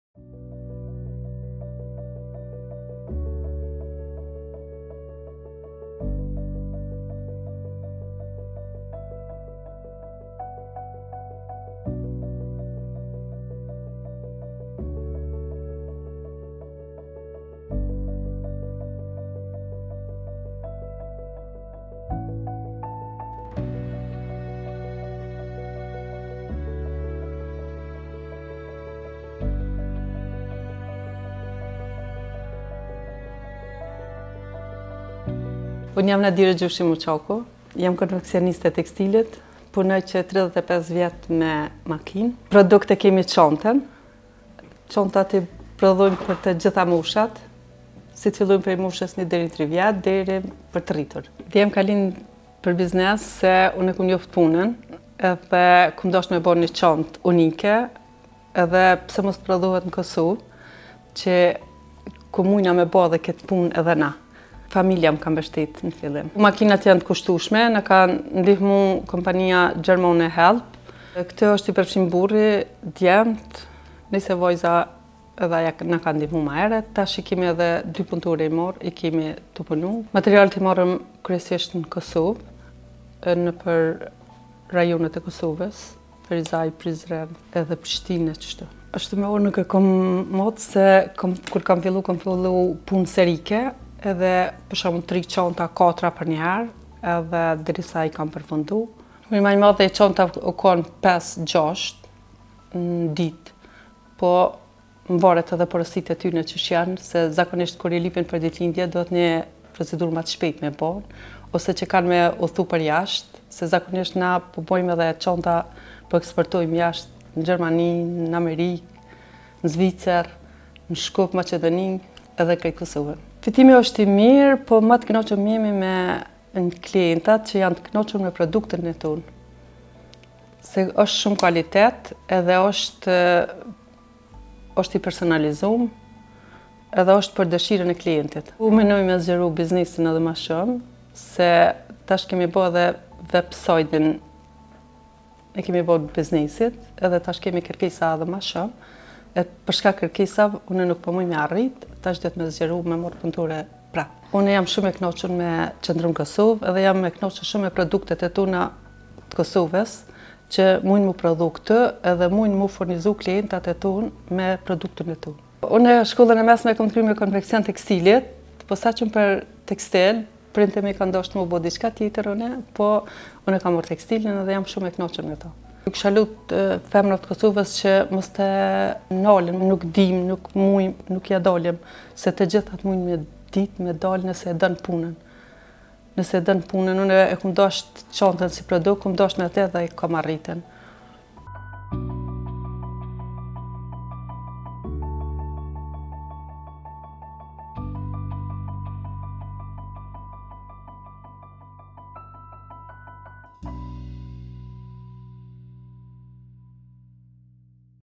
Rrugëtimi i suksesshëm i Nadire Gjoshi Muqakut, me prodhimin e çantave
Rrugëtimi i suksesshëm i Nadire Gjoshi Muqakut, me prodhimin e çantave arriti të depërton përtej kufijve të Kosovës